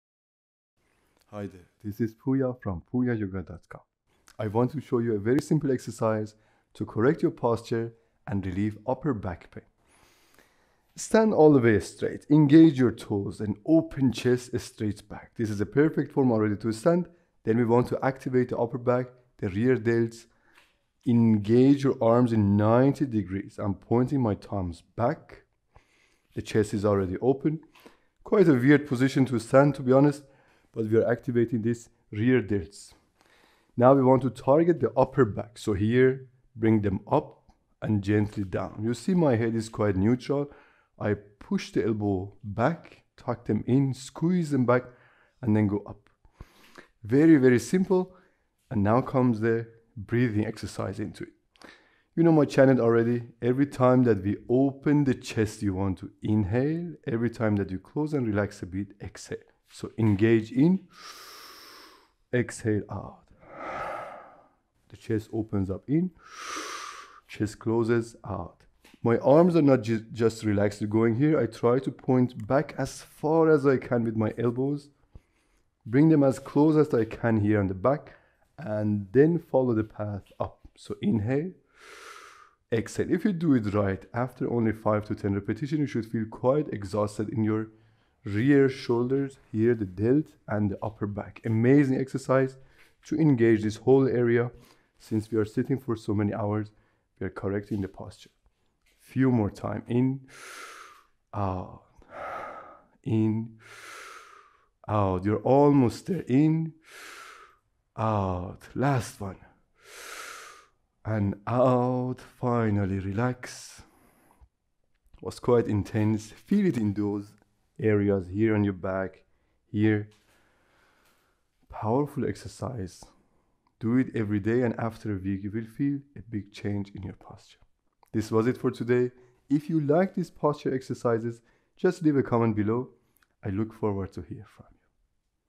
Upper Back Pain Relief Exercise (Posture Correction)
Doing a simple exercise like this daily can already make a huge difference.
Because of our modern lifestyle it's very common to have tension built up in the lower back.
This exercise helps release this tension for instant pain relief and posture correction.